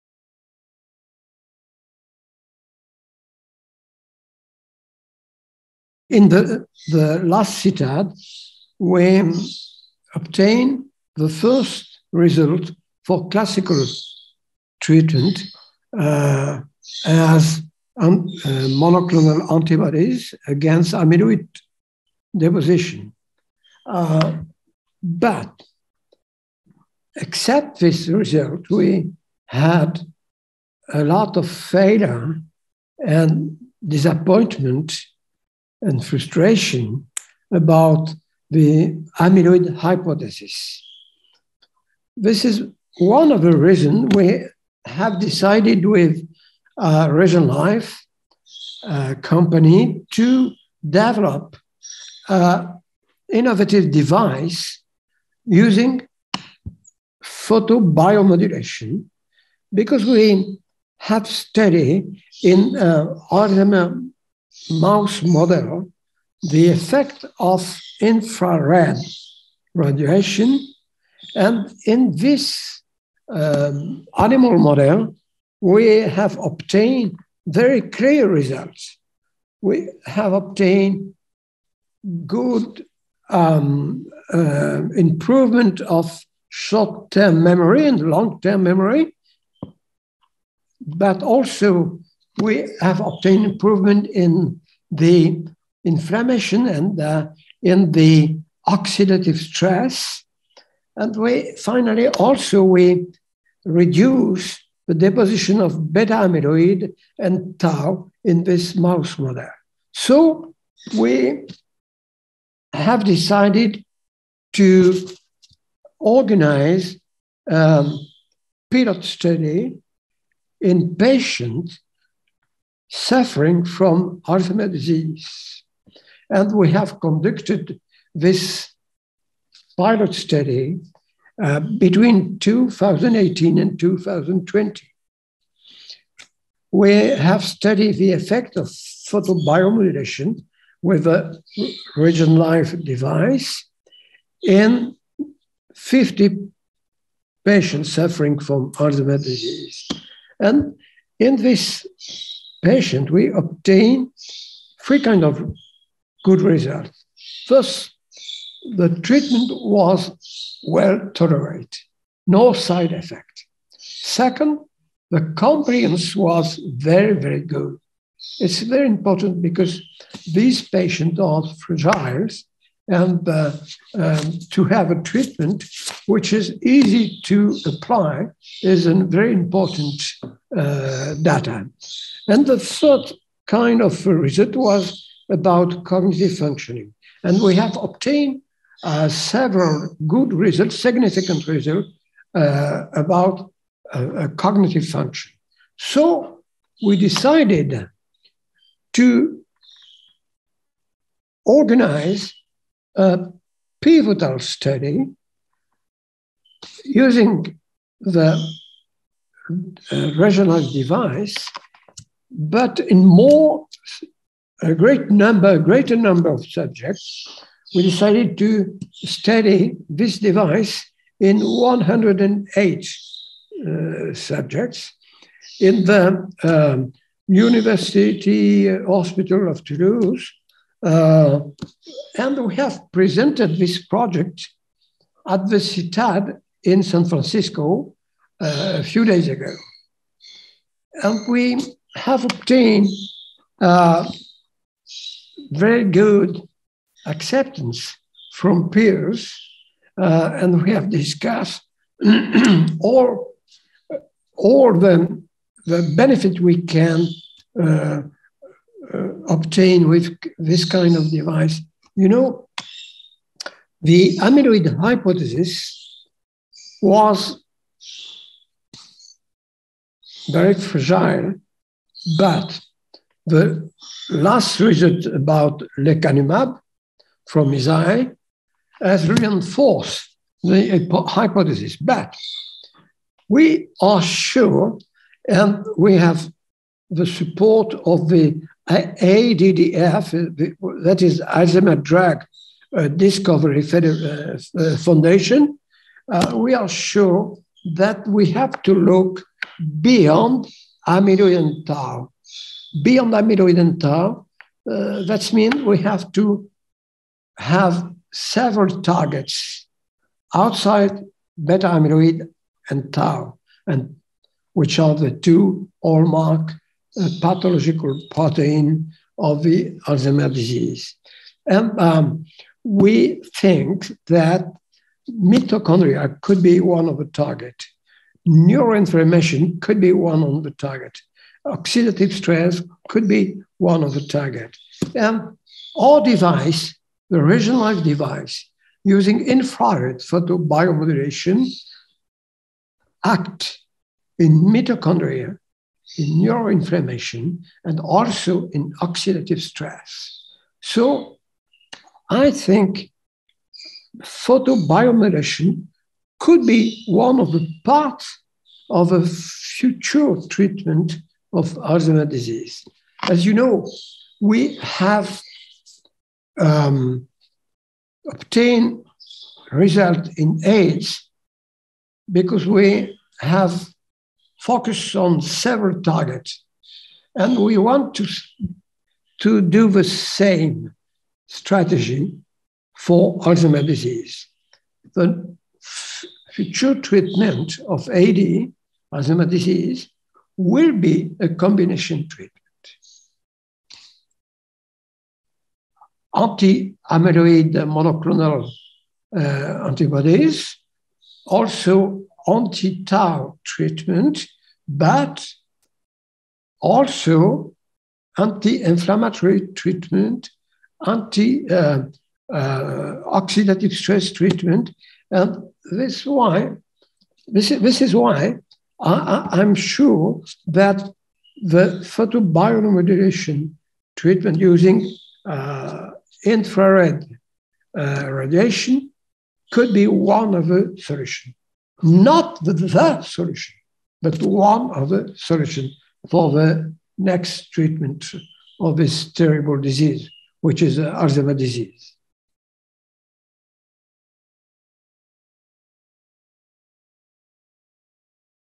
New pivotal study using photobiomodulation in Alzheimer’s treatment
Photobiomodulation (PBM) therapy, previously known as low-level laser therapy (LLLT), is the application of light – typically in the red and near-infrared spectra – to promote tissue repair and reduce inflammation. PBM therapy primarily targets the cytochrome C complex and can increase ATP production, modulate reactive oxygen species, and restore normal cellular function therefore preventing apoptosis. Jacques Touchon, MD, PhD, University Hospital of Montpellier, Montpellier, France, explains how PBM, specifically brain-gut PBM, is being investigated in the treatment of Alzheimer’s disease (AD). Preliminary data from mouse models demonstrated that PBM improved long- and short-term memory and normalized levels of amyloid-β 42, tau, oxidative stress, and neuroinflammation. This data was used to support a randomized, double-blind, sham-controlled pilot study in which 53 patients with mild-to-moderate AD were enrolled. PBM was administered using the REGEnLIFE RGn600 medical device and was concluded to be safe and well tolerated (44% of patients experienced adverse effects and this was balanced between trial arms). Prof. Touchon presents the design of a new, larger study, named LIGHT4LIFE, which will investigate the efficacy of PBM therapy on patient cognition as measured using the ADAS-Cog score. This interview took place at the Clinical Trials on Alzheimer’s Disease Congress 2022 in San Francisco.